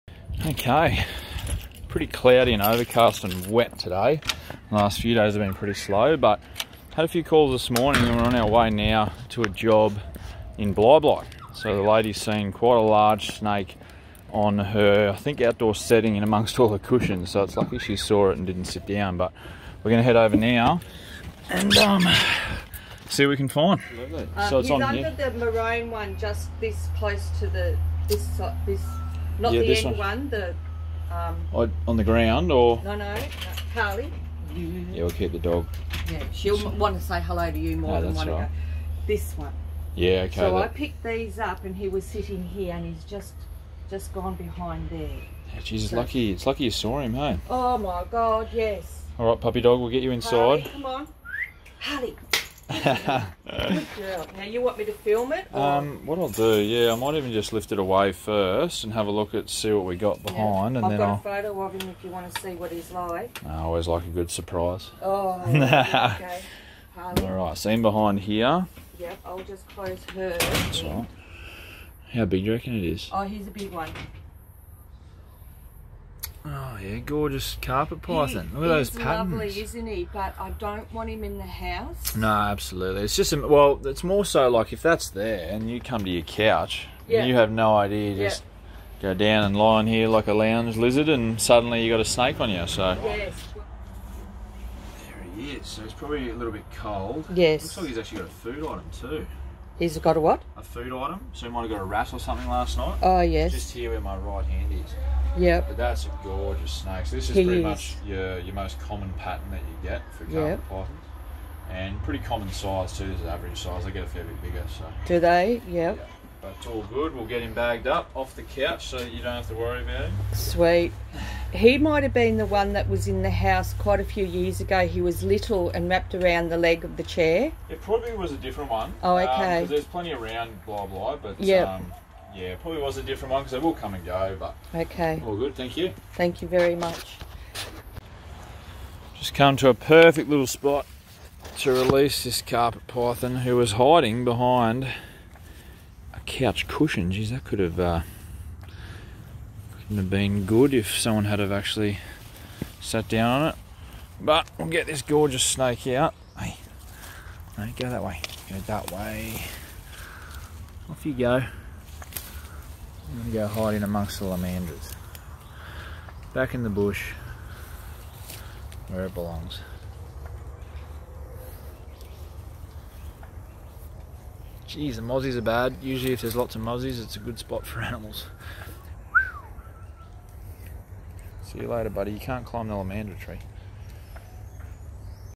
Snake Hiding Behind Cushion On Couch!!
It's lucky the resident at a home in Bli Bli this morning saw this Carpet Python go behind the cushion or she may have been in for a big surprise.

It was very comfy chilling out on the couch behind the cushion and I am sure it would have slept there all day. The lady had pets and didn't want them having a confrontation so she preferred the snake relocated to a nice patch of bushland.

- Stu